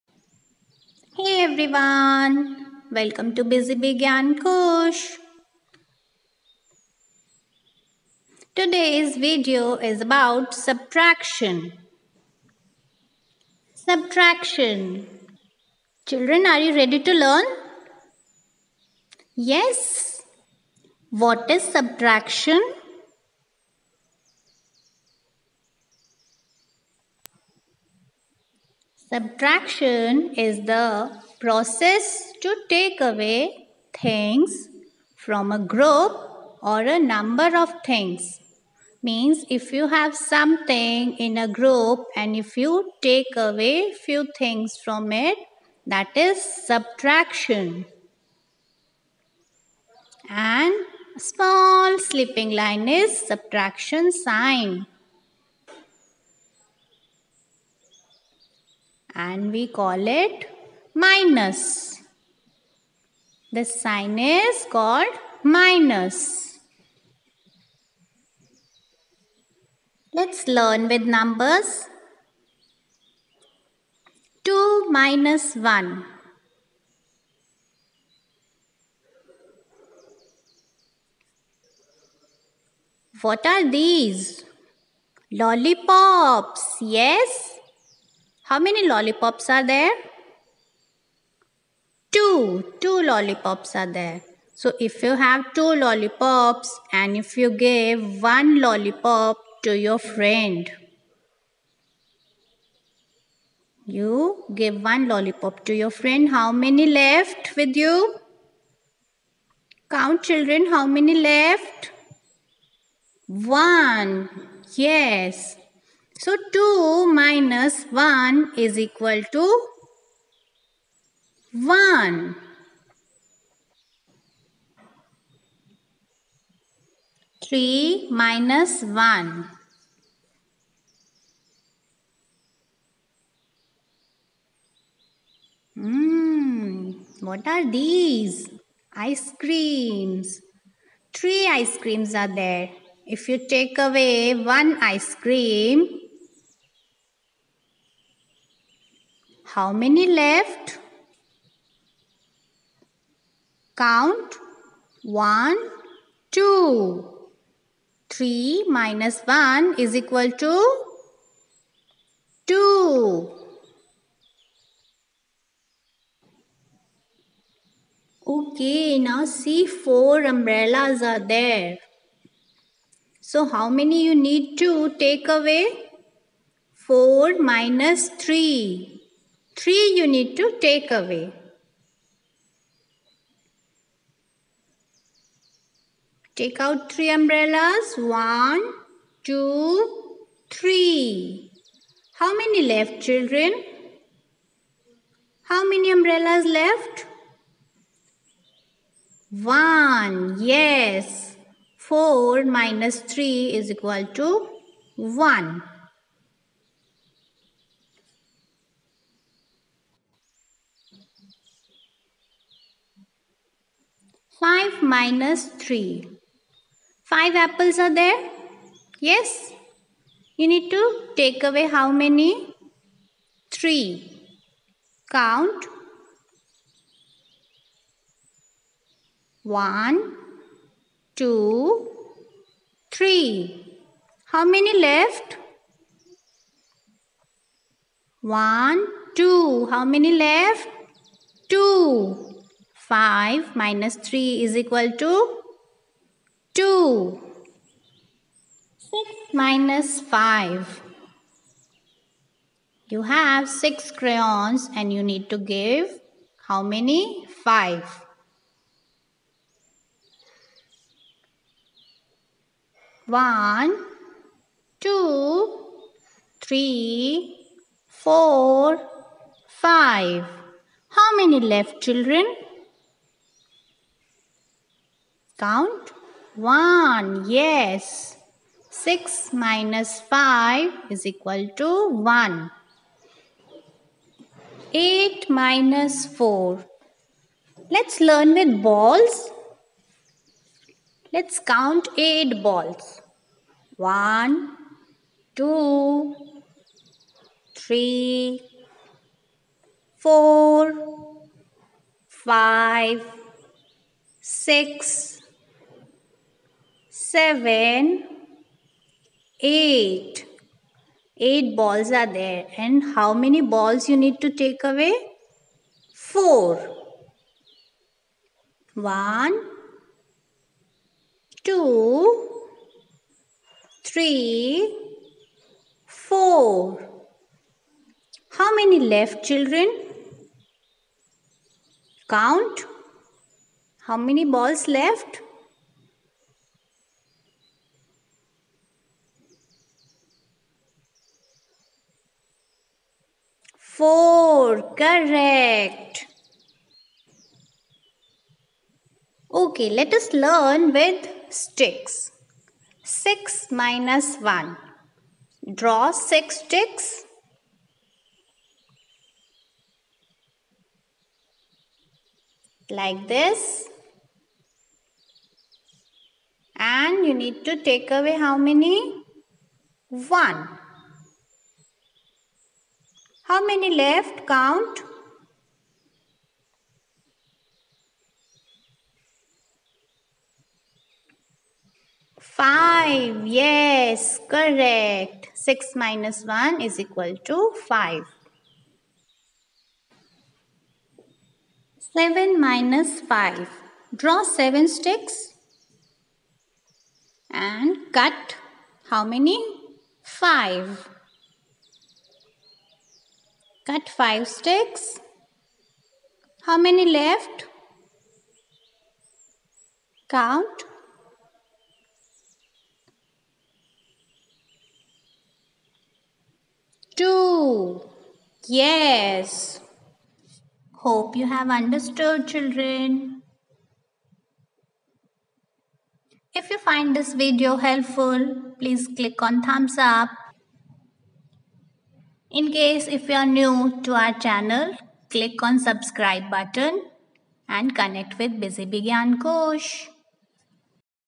SUBTRACTION For Kids | subtraction for UKG
Hello Everyone,

This video is for kids who is learning subtraction for the first time.
Colourful visuals will help them to understand and learn faster.
This will motivate me to make more interesting  educational videos.

Warm Regards,
busybeegyankosh